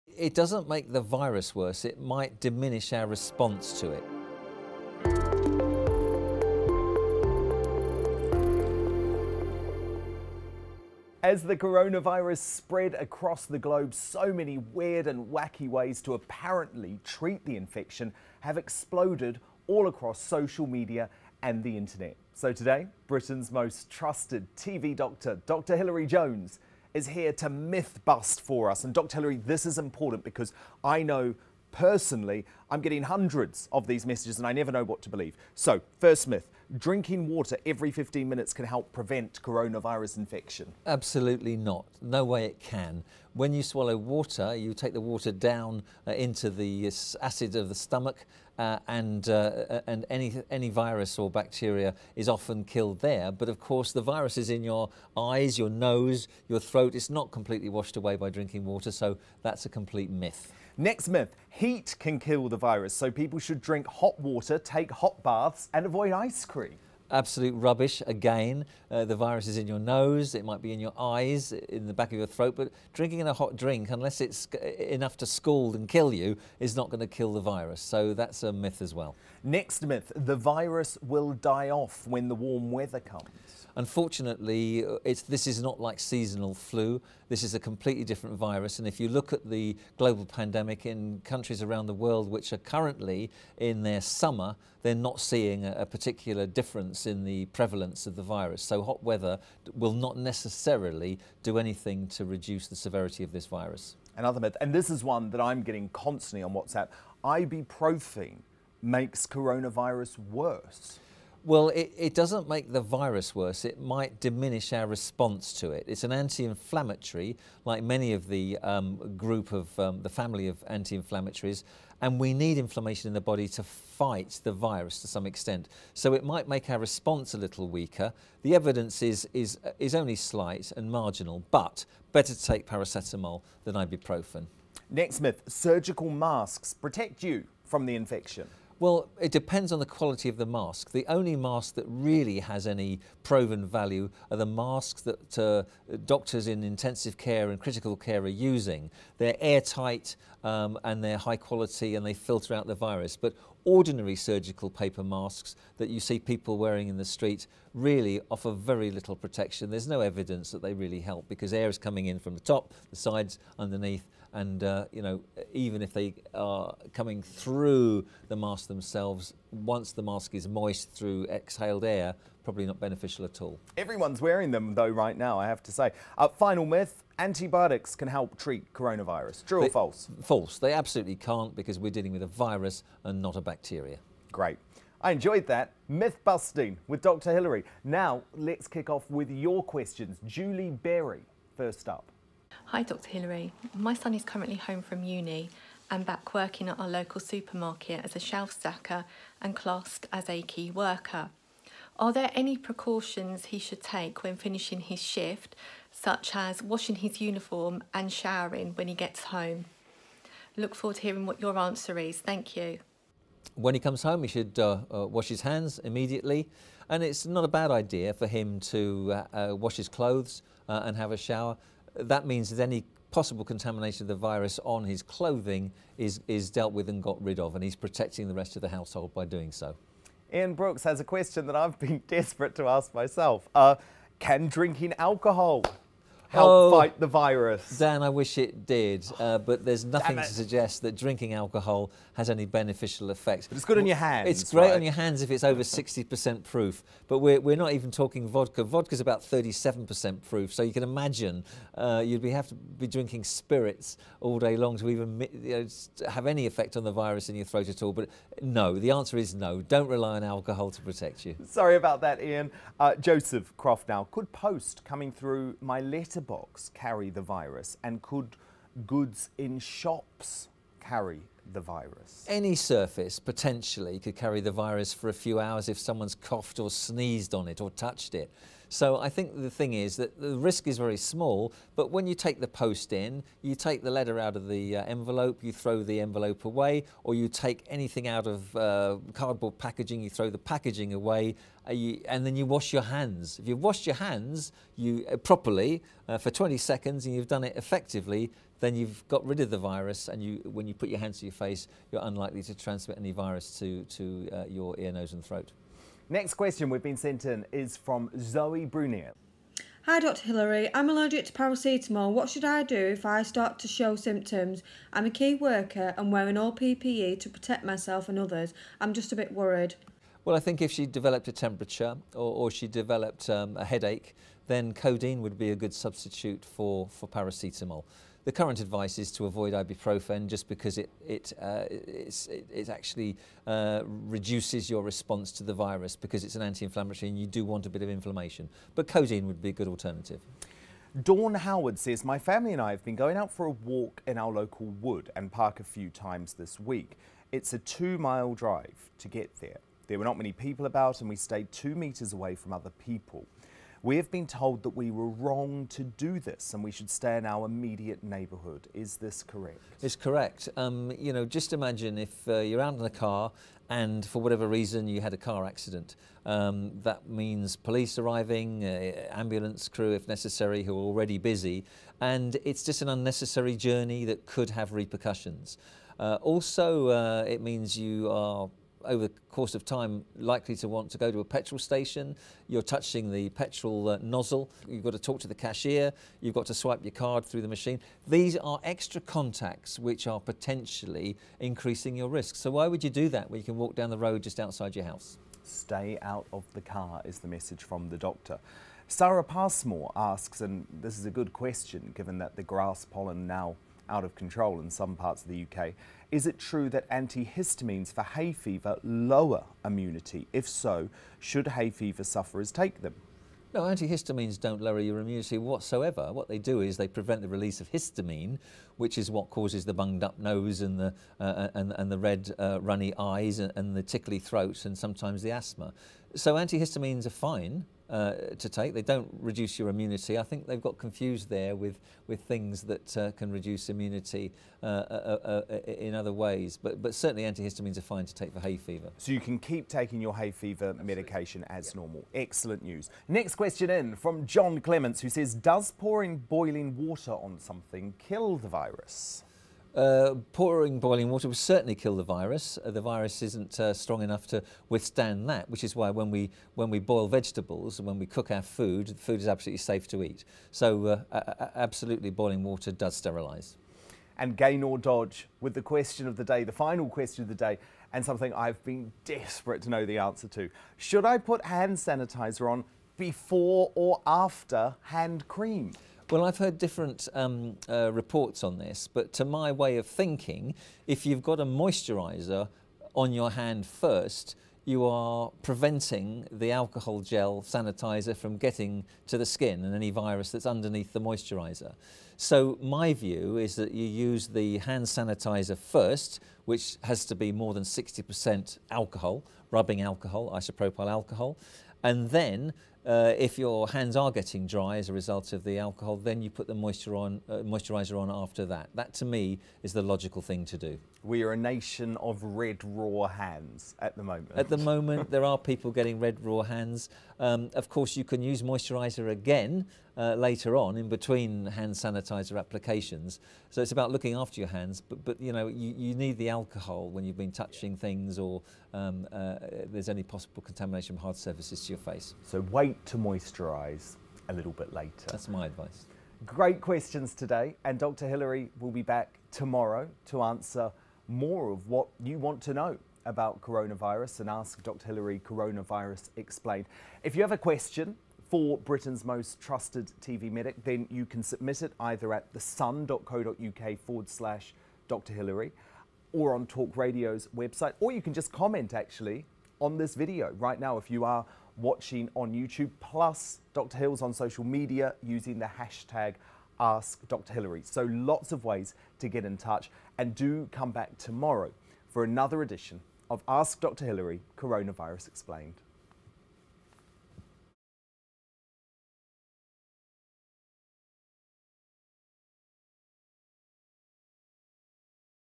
Coronavirus explained: COVID-19 myths busted and your questions answered
As coronavirus spreads around the globe, unusual ways to prevent infection have been widely circulated on social media. Today we bust the COVID-19 myths with Dr Hilary Jones, including:

- Can drinking water every fifteen minutes can help prevent infection?
- Does drinking hot water, taking hot baths and avoiding ice cream help?
- Will coronavirus die off in warmer weather?
- Does ibuprofen make coronavirus worse?
- Can surgical masks protect you from infection?
- Can antibiotics help treat coronavirus
- Can drinking alcohol help fight the virus?

Dr Hilary also took on Sun reader questions...

- Could post coming through my letterbox carry the virus?
- I am allergic to paracetamol, what should I do if I should start to show symptoms?
- Should my family and I stay in our immediate neighbourhood to exercise?
- Is it true that antihistamines for hay fever lower immunity?
- Does pouring boiling water on something kill the virus?
- Should I put hand sanitiser on before or after hand cream?

Dr Hilary will be here every Monday - Thursday to answer your coronavirus questions.
Submit them in the comments with the hash tag